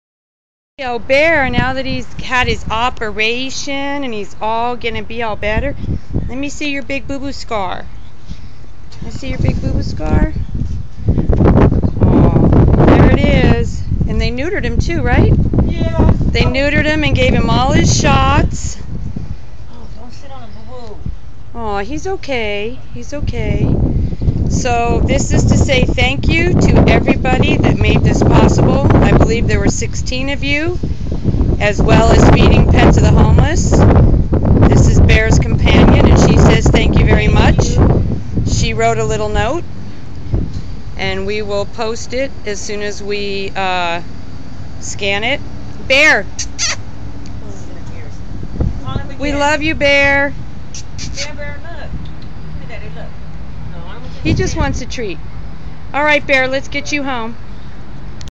"Bear" Leaves VanBuren Animal Hospital 5-22-13
Donations from 16 people as well as help from Feeding Pets of the Homeless. "Bear" was finally able to have surgery for his femur head fracture, get neutered and receive all of his vaccinations. Everything made possible by supporters of Taking It To The Streets with Lori and Shira.